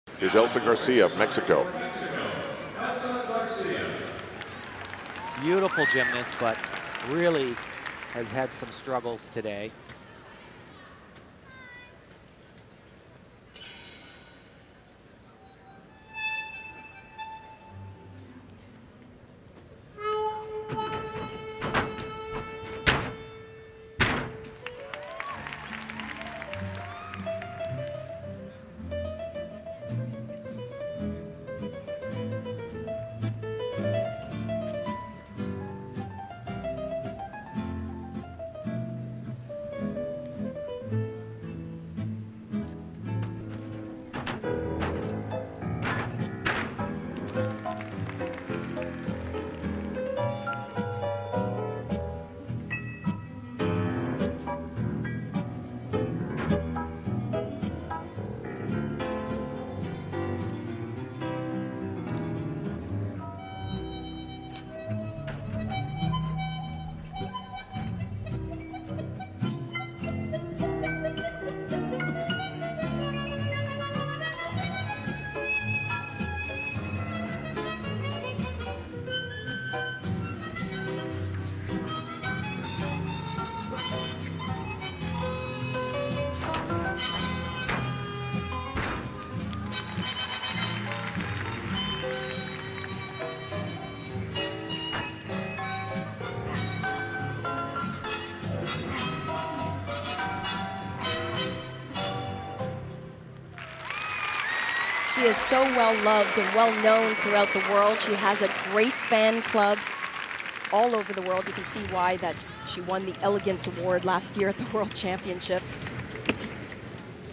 Elsa Garcia - Floor Exercise - 2010 Tyson American Cup
Score: 13.800 - Elsa Garcia (Mexico) - March 6, 2010 - DCU Center - Worcester, Mass.